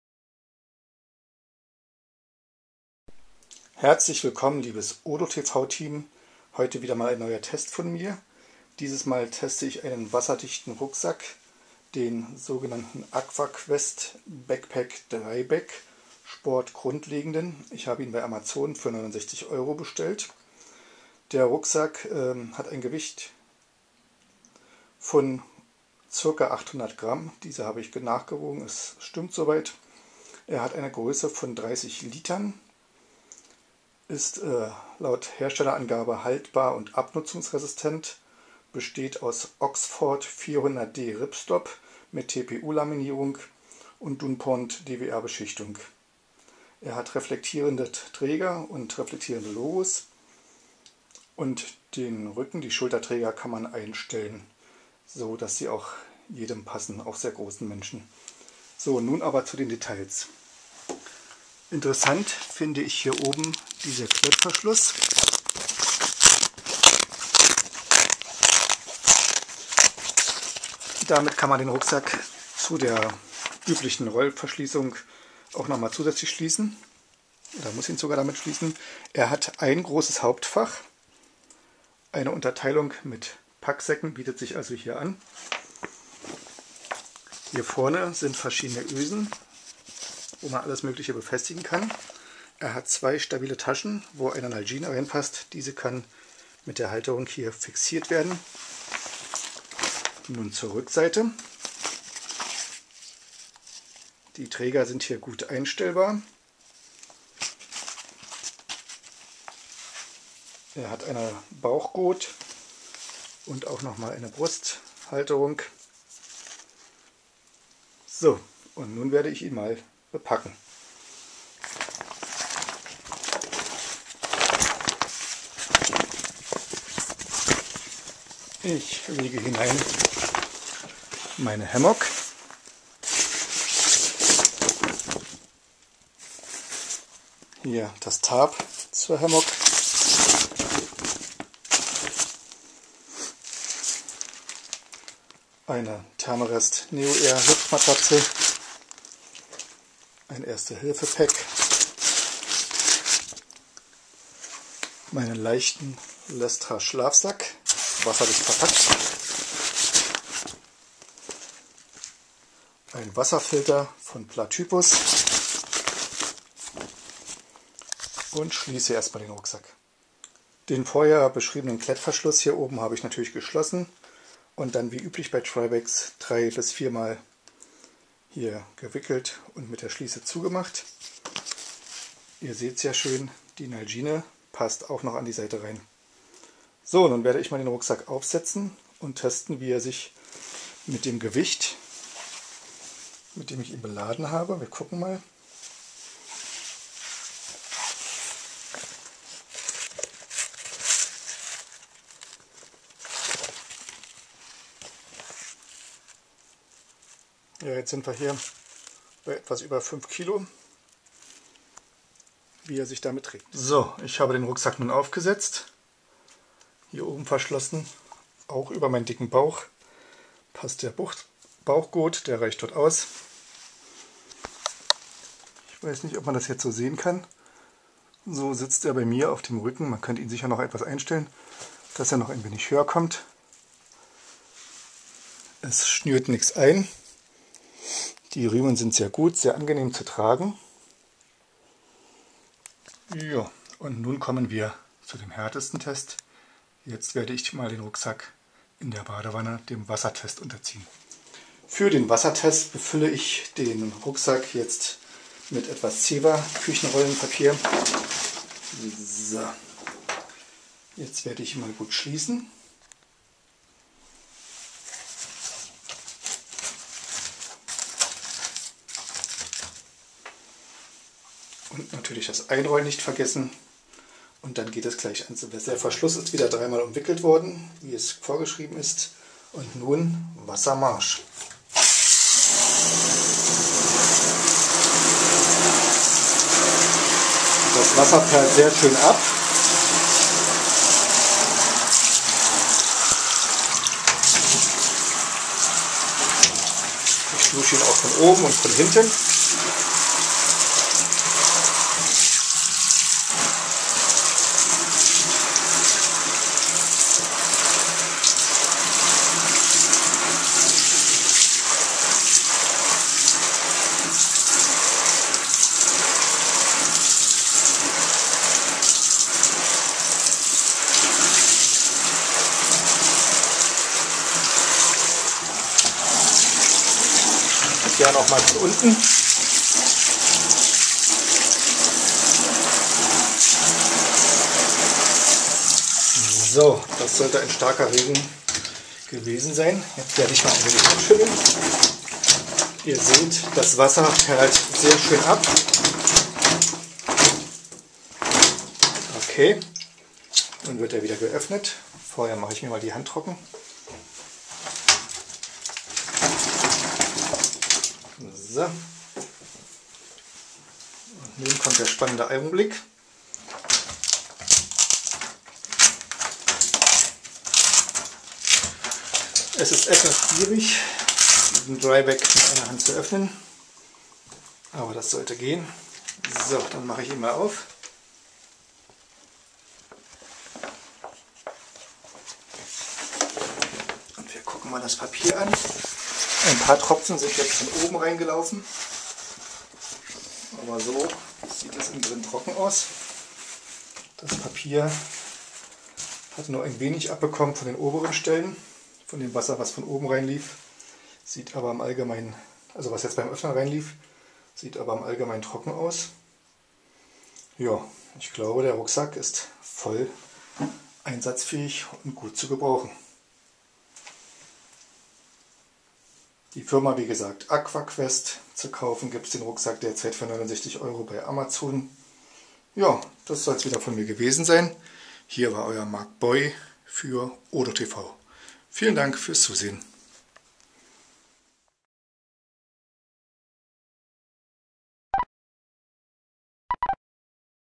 Aqua-Quest_DryBag Test
30 L DryBag aus Kanada von der Firma Aqua-Quest